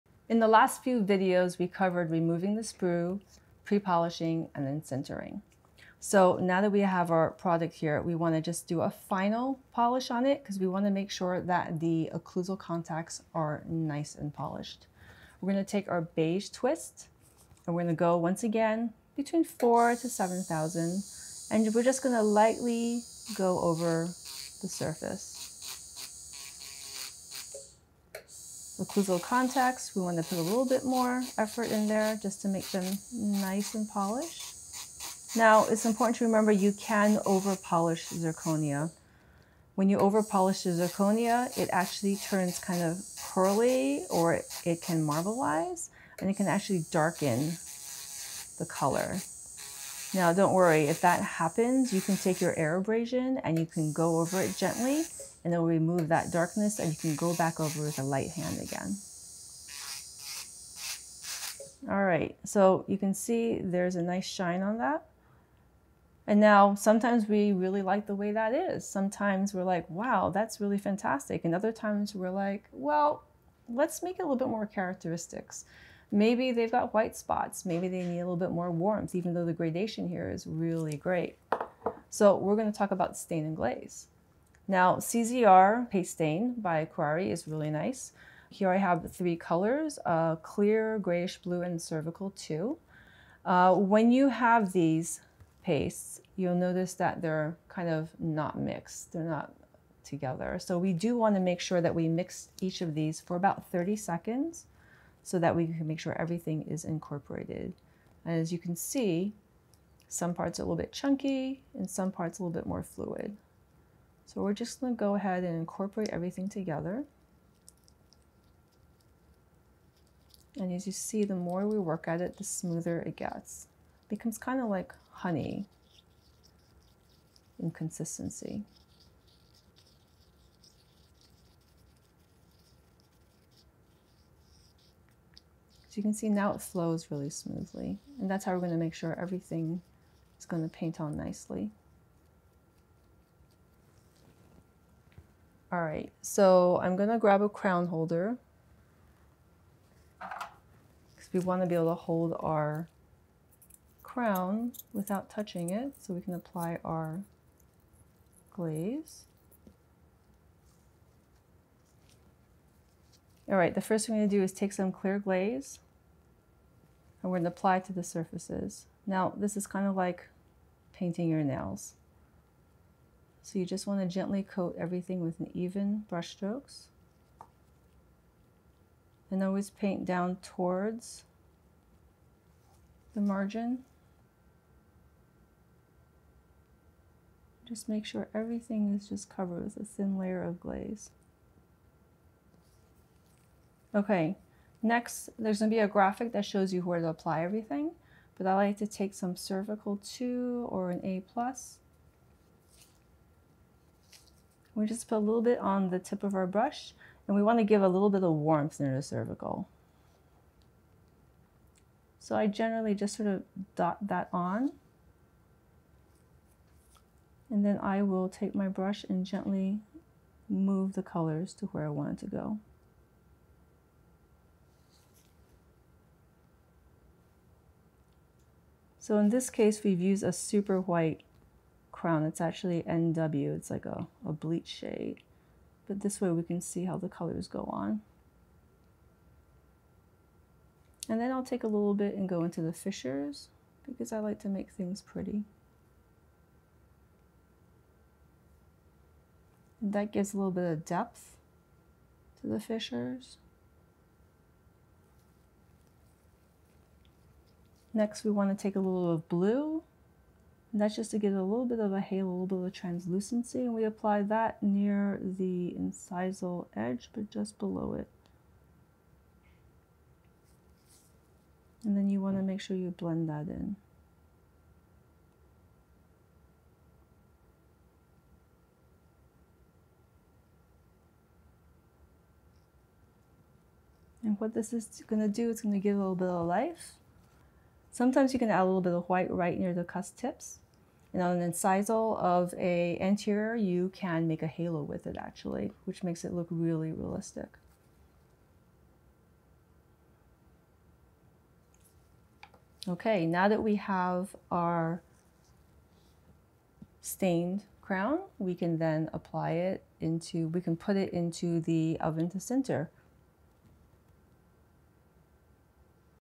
Zirconia 101: 8 of 11: Post sinter polish and stain glaze
Join us on a Journey into the World of Zirconia with Dr. Penelope Chang: Part 8 of 11.

This video covers post-sintering polishing and characterization of a KATANA Zirconia crown. It demonstrates using a beige twist polisher to achieve a high shine on the occlusal contacts while avoiding over-polishing.
The video then introduces staining and glazing techniques, using CZR paste stain and clear glaze. It details mixing the stain paste for smooth application and demonstrates applying clear glaze, cervical 2 stain for warmth, blue stain for translucency, and highlighting details with white and blue. The video concludes by highlighting the final stained crown ready for glazing.